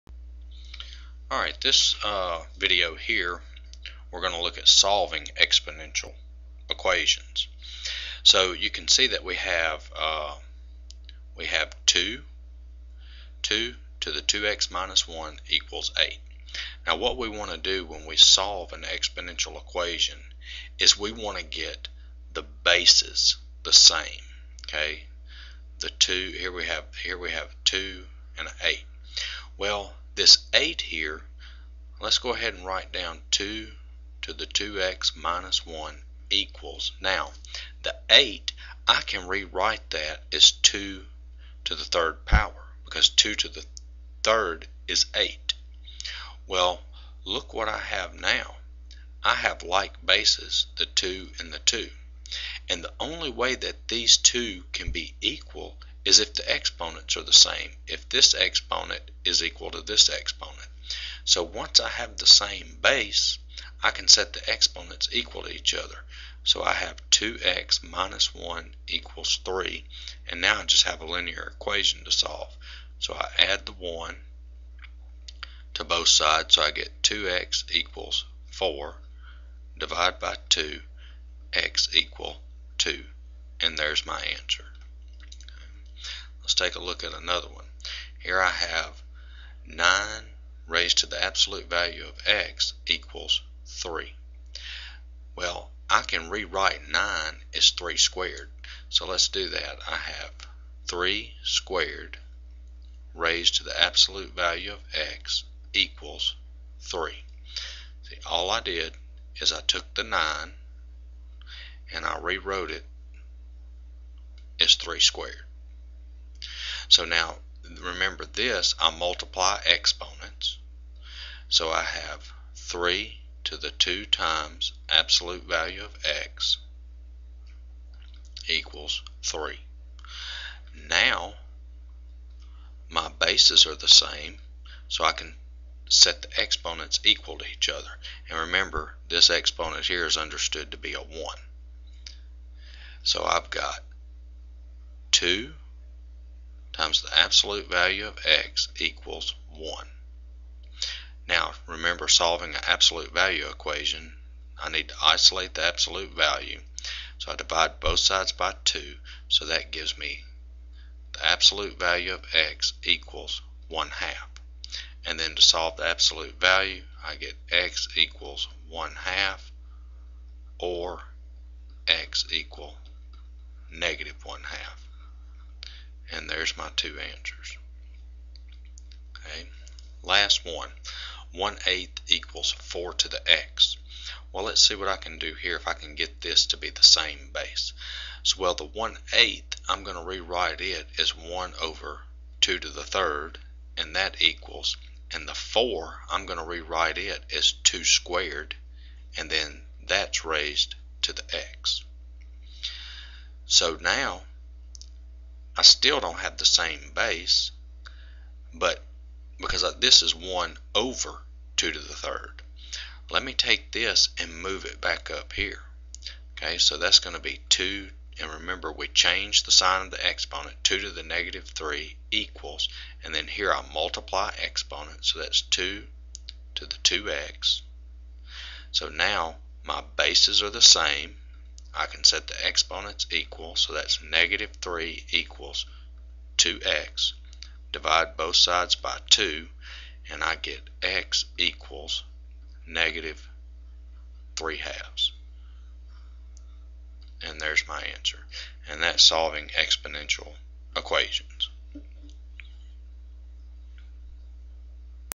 Solving Exponential Equations by Getting Like Bases
This video shows how to solve exponential equations.